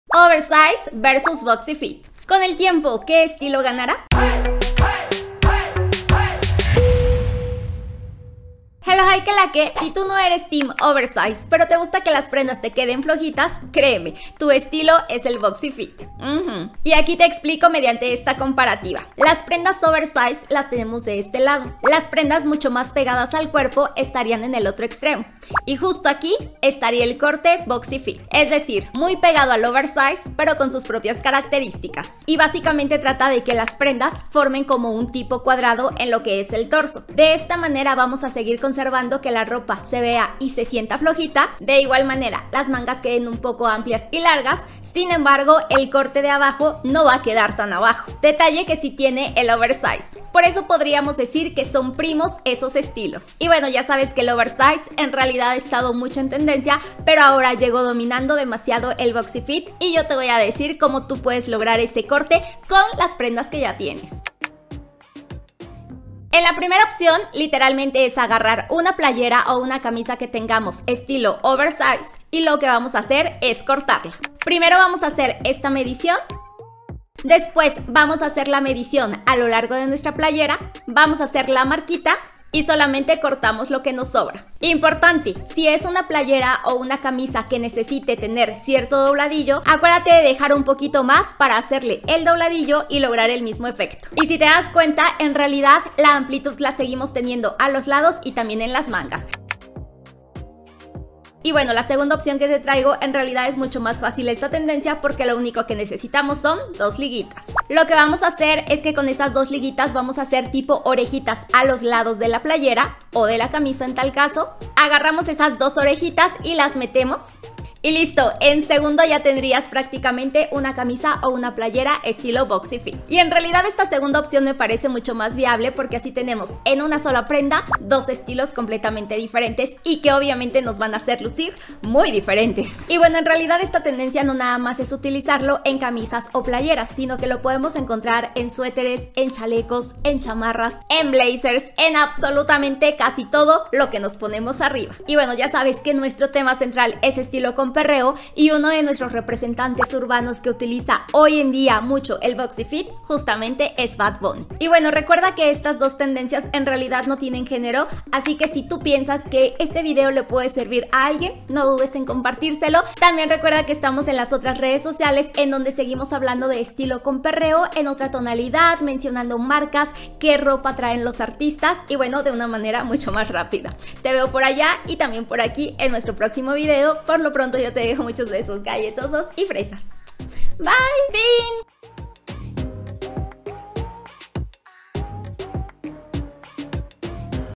✂️ BOXY FIT: Cómo hacer una playera OVERSIZE en BOXY FIT😉
HELLO, HI..😜😁
🎬🎬  En el video de hoy te te digo dos técnicas de cómo transformar una playera oversize que ya tengas en un corte más actual, como lo es el BOXY FIT.

Espero te guste, disfrutes mucho este video, le dejes mucho amor 👍👍👍 y también que lo puedas compartir 😉😉

Te dejo muchos besos galletosos y fresas 😘😘😘😘